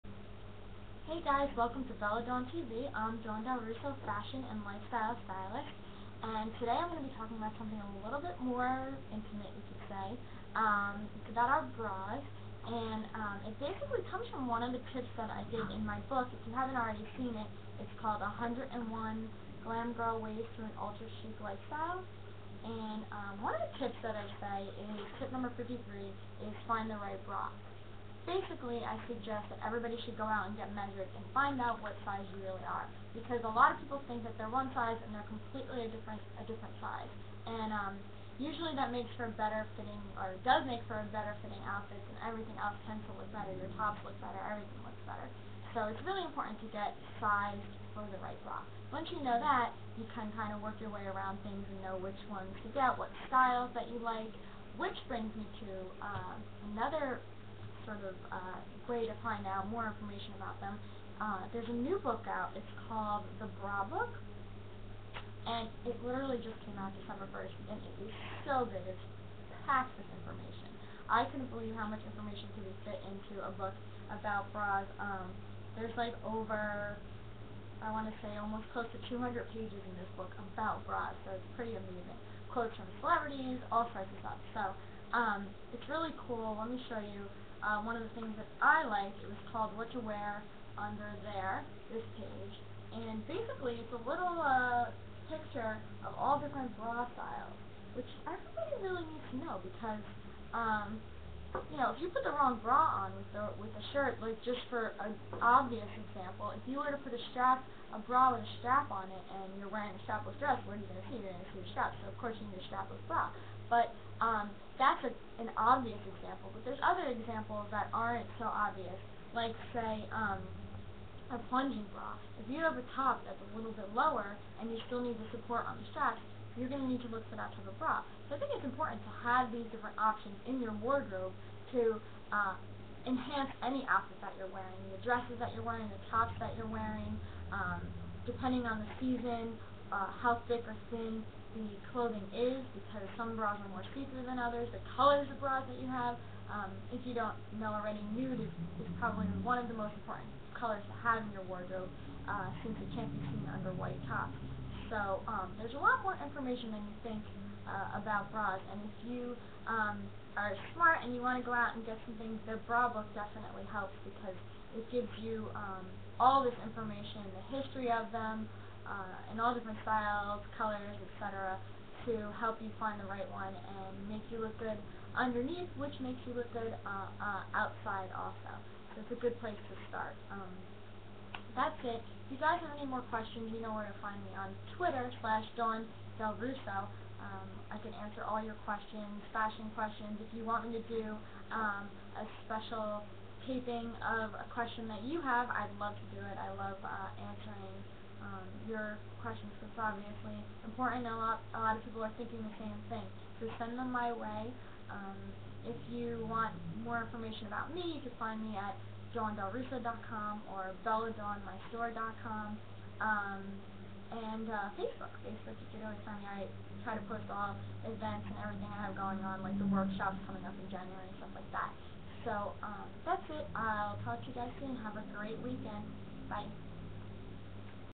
How to Find the Right Bra Features "The Bra Book" - Fashion Style Tips
Dawn Del Russo fashion and lifestyle stylist talks about finding the right bra, and getting fitted base on tip #53 from her book "101 Glam Girl Ways to an Ultra Chic Lifestyle". Features "The Bra Book"  Dawn has been seen on Fox 5 NY for her fashion tips and loves answering questions on twitter @dawndelrusso.
Ep.7